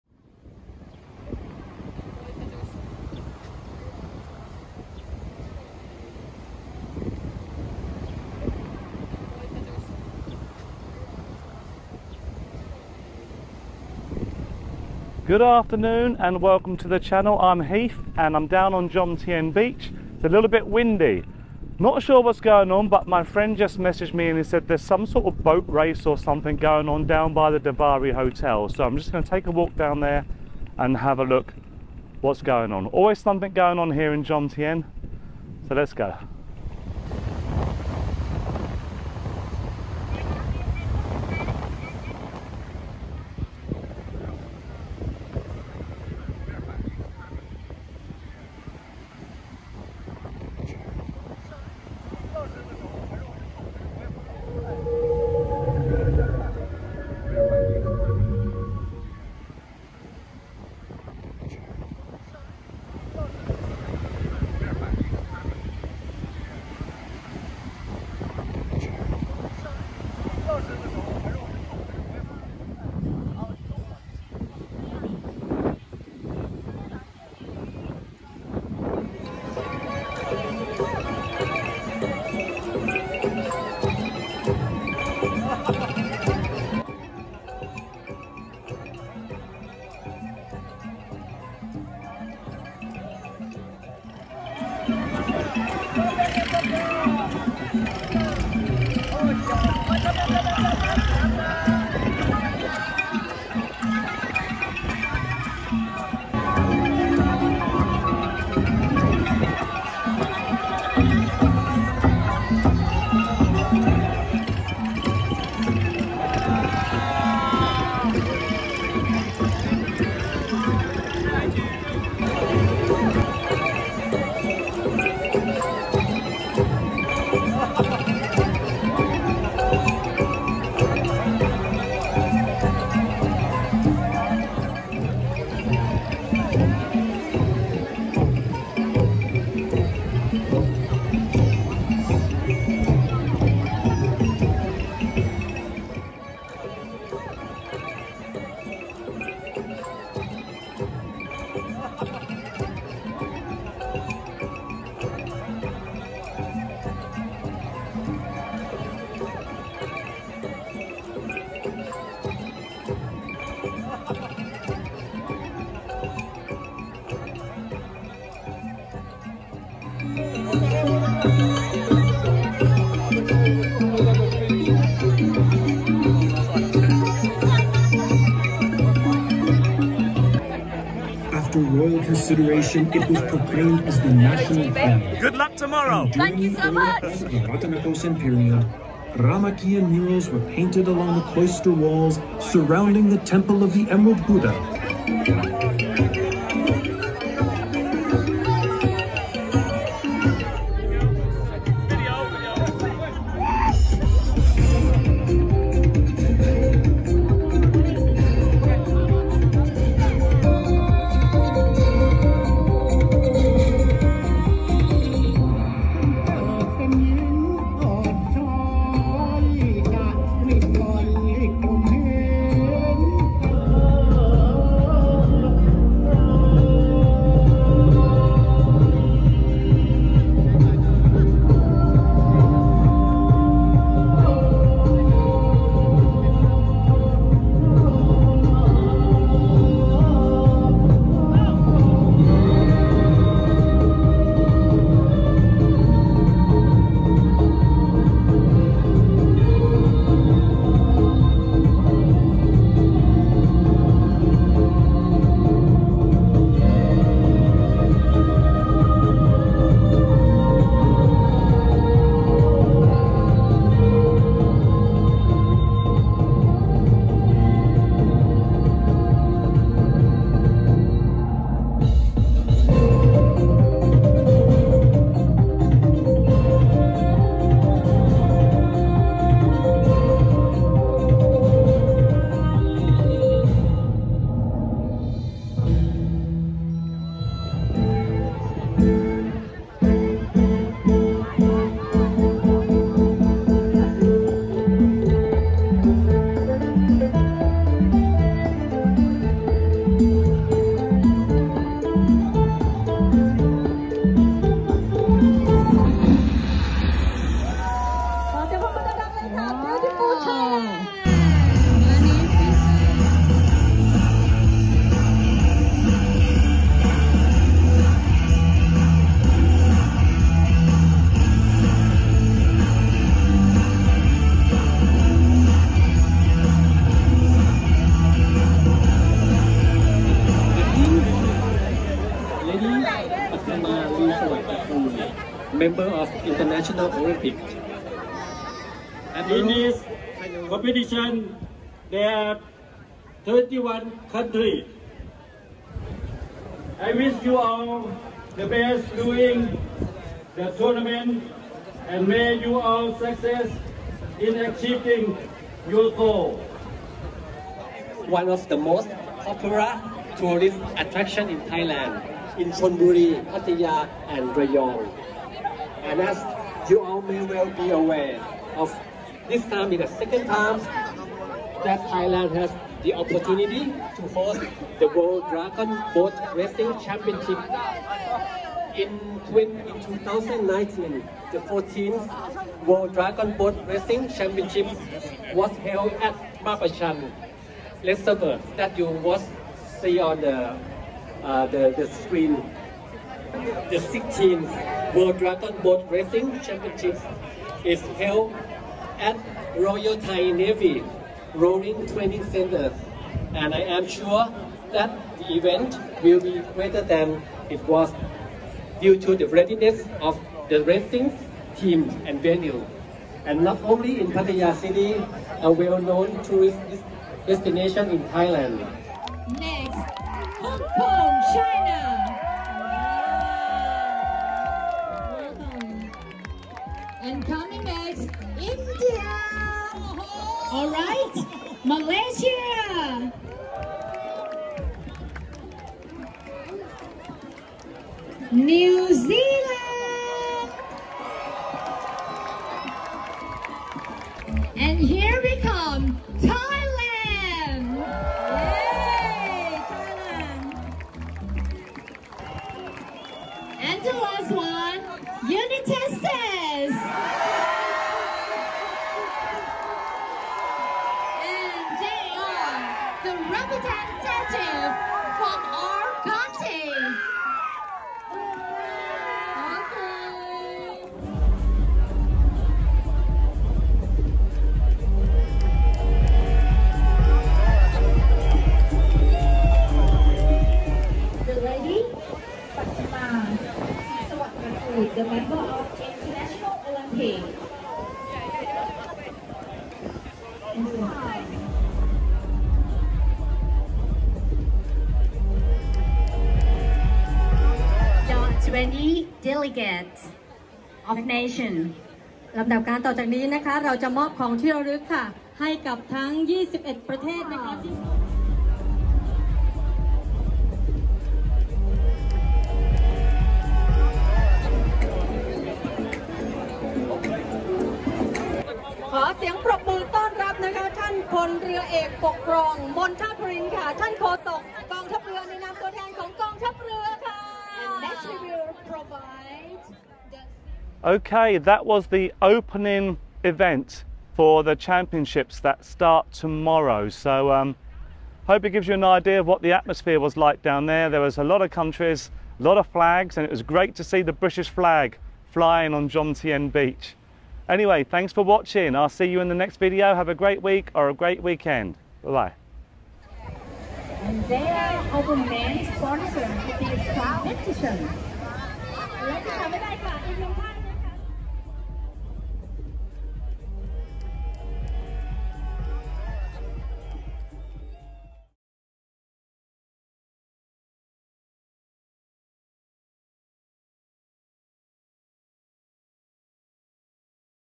Jomtien Pattaya Beach. Dragon Boat Racing Championships Opening Ceremony 2023
Jomtien Pattaya Beach and the opening ceremony to the 16th IDBF World Dragon Boat Racing Championships 2023. Thailand has hosted the championships before in 2019. There were 31 countries competing and many people on the beach. The competition is held at the Royal Navy Rowing and Canoeing Association Center in Rayong, Thailand.

The event is normally every 2 years but it was cancelled in 2021 due to Covid. The 2023 championships were going to be hosted in Hong Kong but this was cancelled due to Hong Kong still having Covid restrictions in place. So the event was hosted once again by Thailand.

The host countries supply the boats for all teams.

You can give us a SuperThanks (I think this only appears under the video on a desktop)

Thank you. Have a great week or a great weekend. 😎